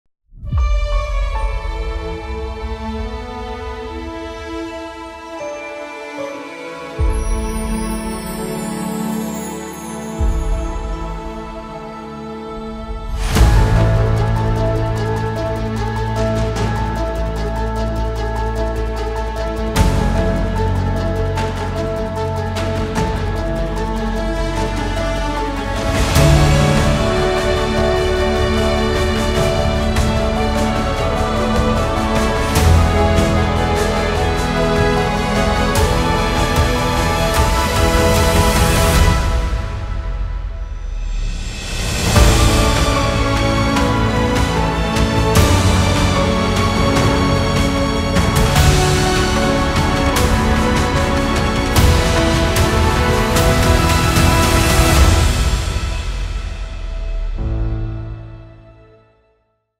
Буктрейлер по книге Виктора Дашкевича «Граф Аверин. Колдун Российской империи»
Первая книга серии о расследованиях графа Аверина.